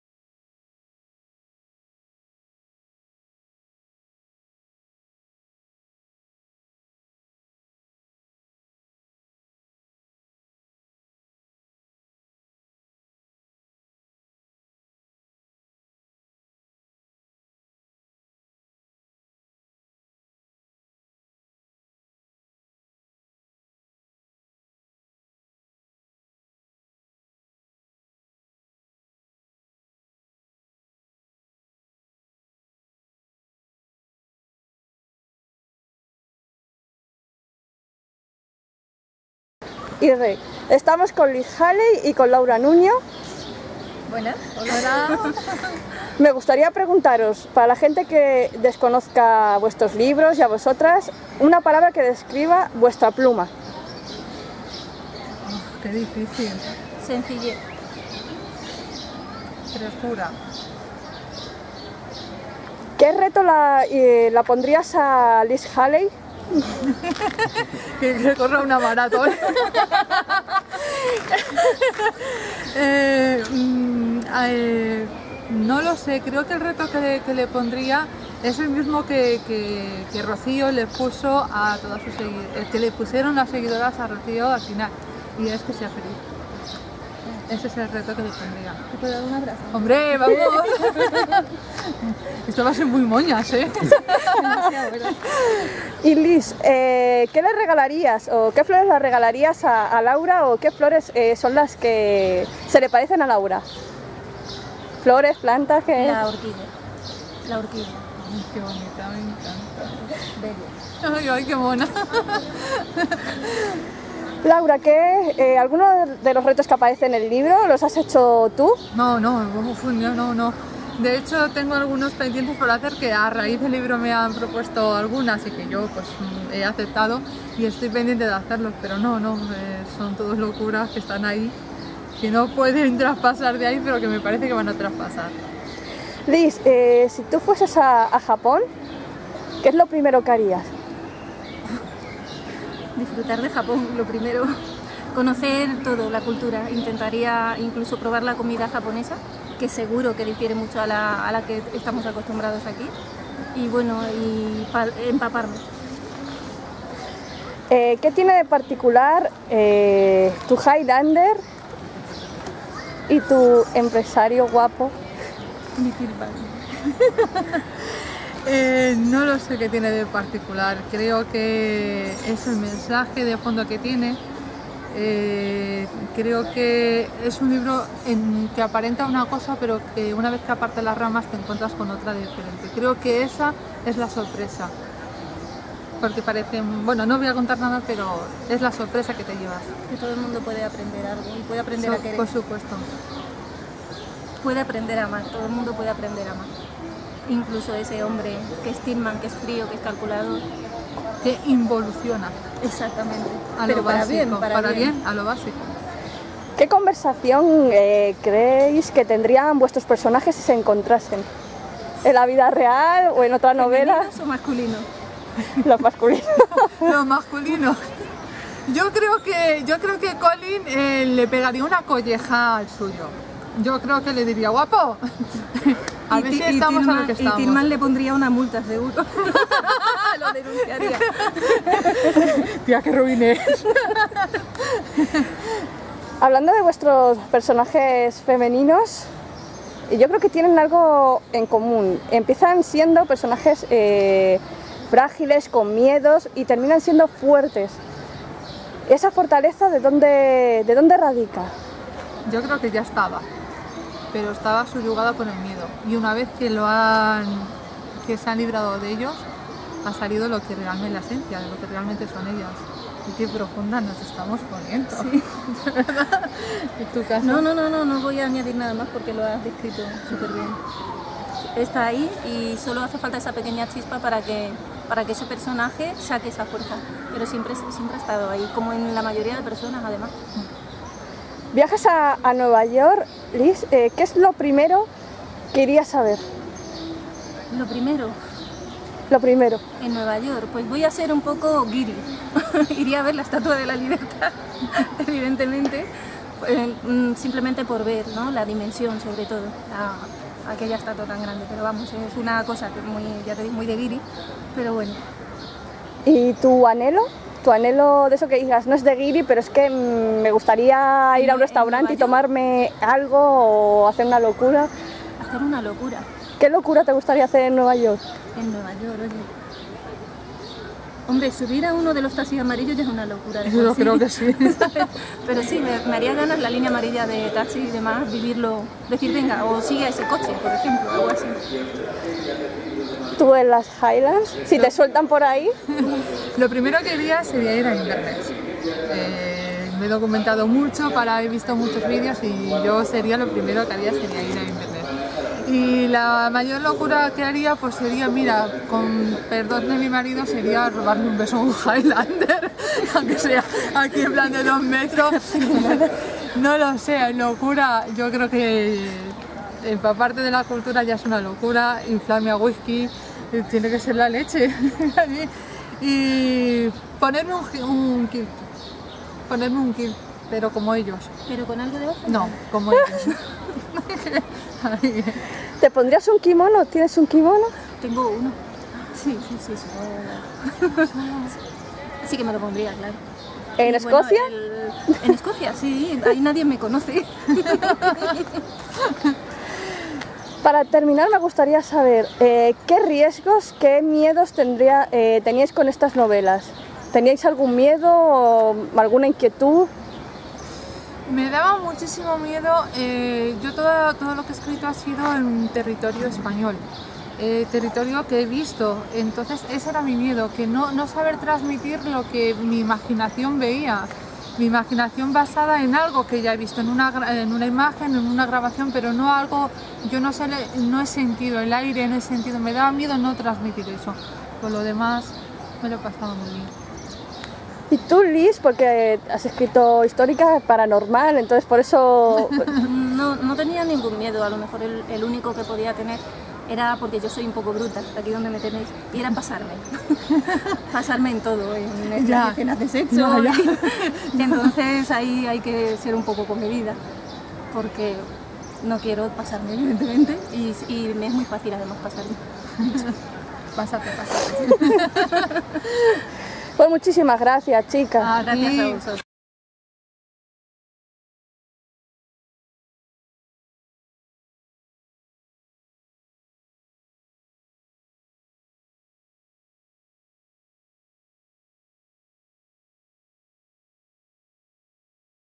Unos mínutos con Laura Nuño&Lis Haley
Nos acercamos a la Feria del Libro 2015 y nos citamos con Laura y Lis, ambas hace poco han sacado su libro con Cristal.
Laura Nuño "Highlander tenias que ser" y Lis "Un millar de flores"